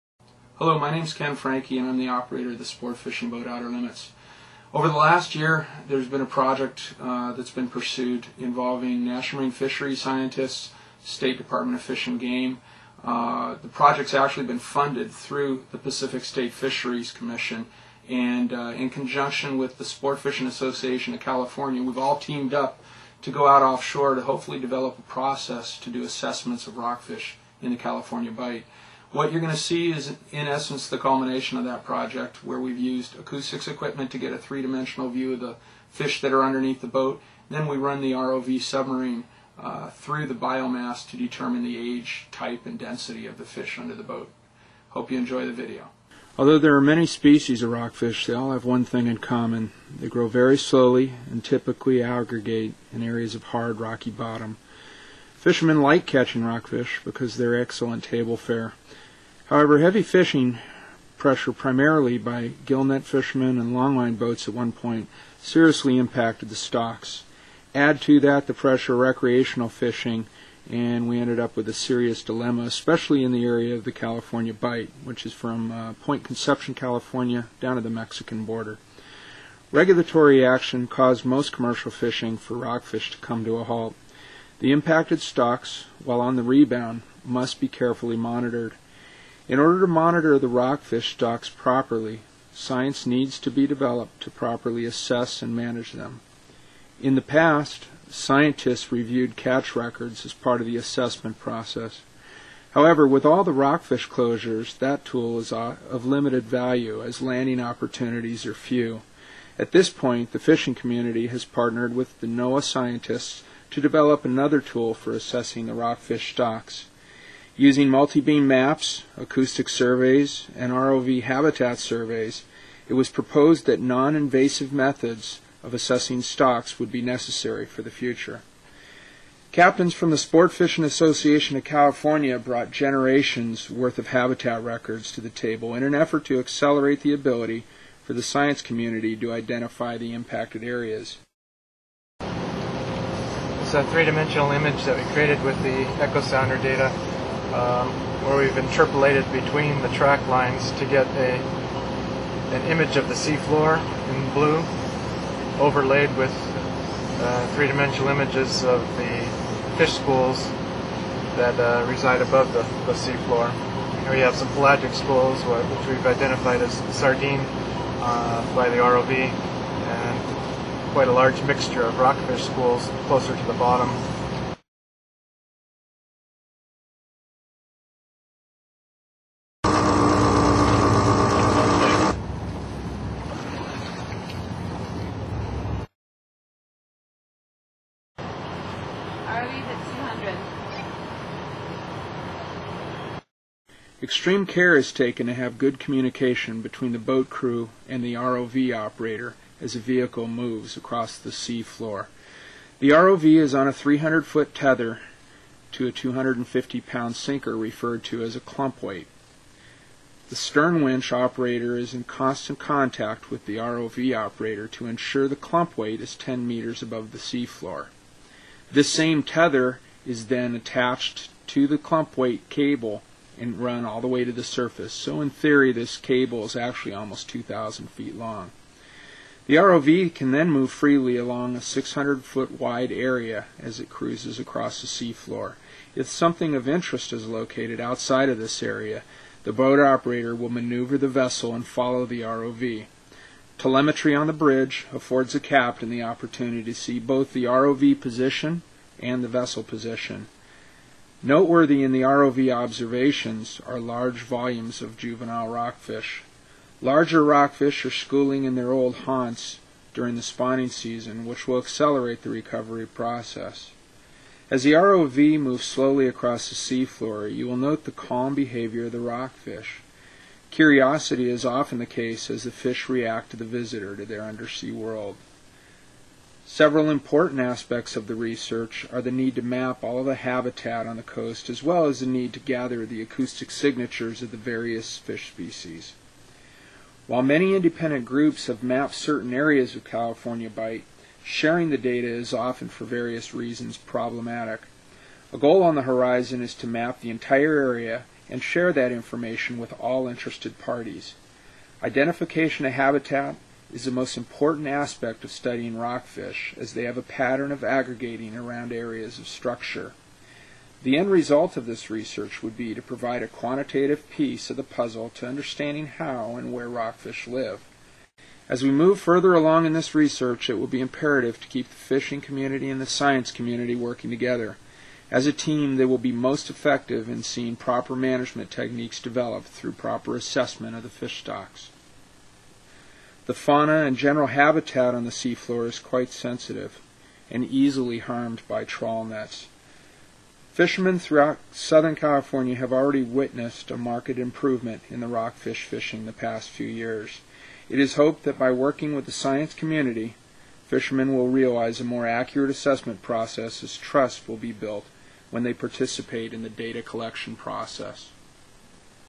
Acoustic and ROV Survey Techniques: Part I
From Point Conception to the Mexican border, government scientists and recreational fishermen work cooperatively to map rockfish habitat and identify species using passive acoustic methods and underwater video.